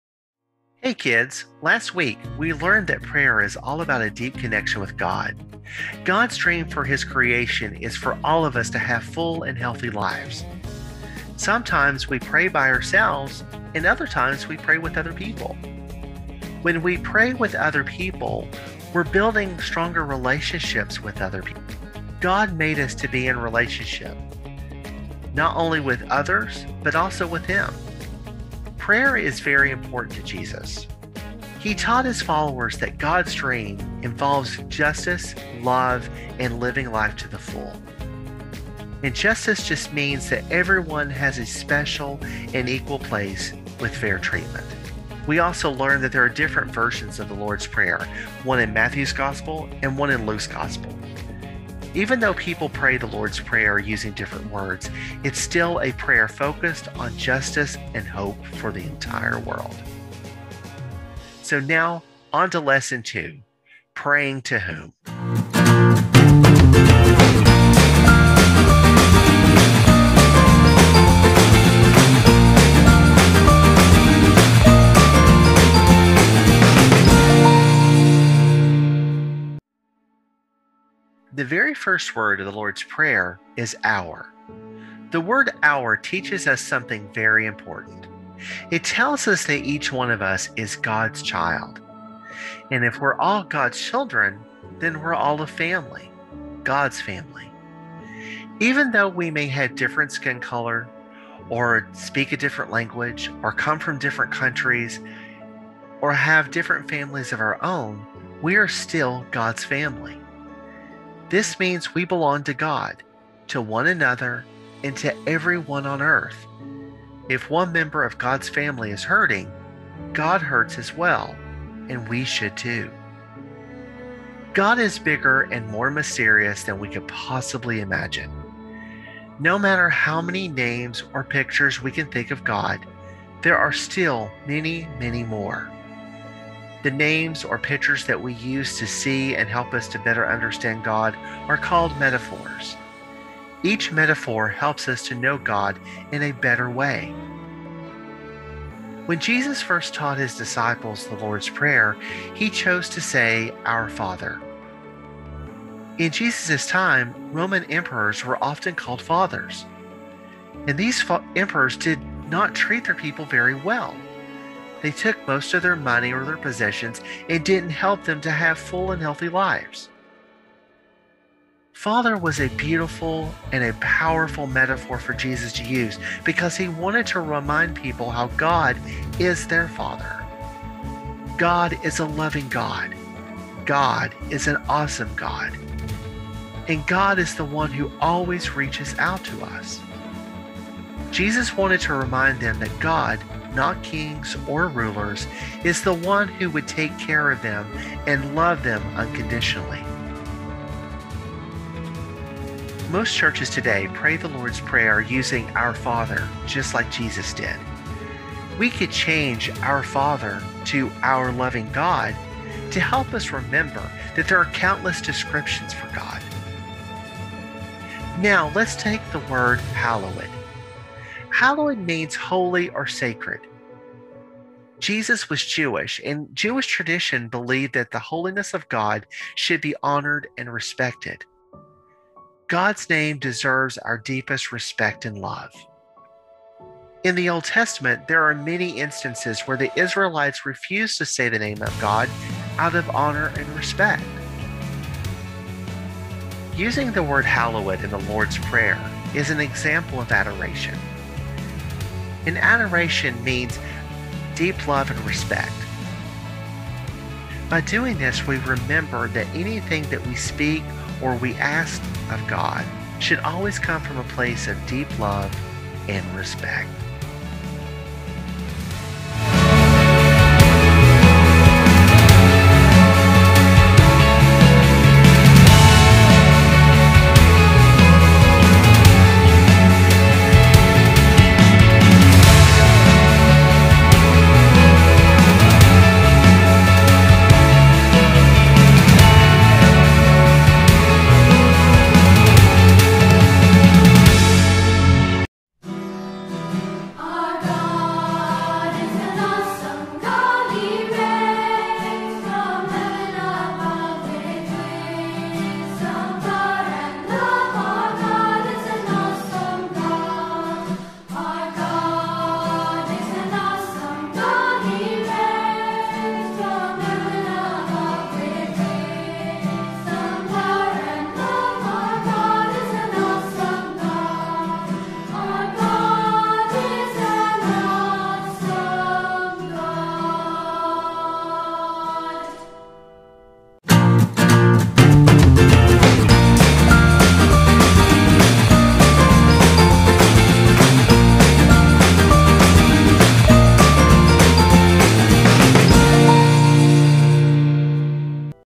St. Alban's Online Sunday School, Fall 2021 - The Lord's Prayer - Praying to Whom?
We will learn about the names and attributes of God, adoration in prayer, and how the opening of the Lord’s Prayer connects all of us with God and each other.

Accompanying lesson materials and activities are located in the Narthex.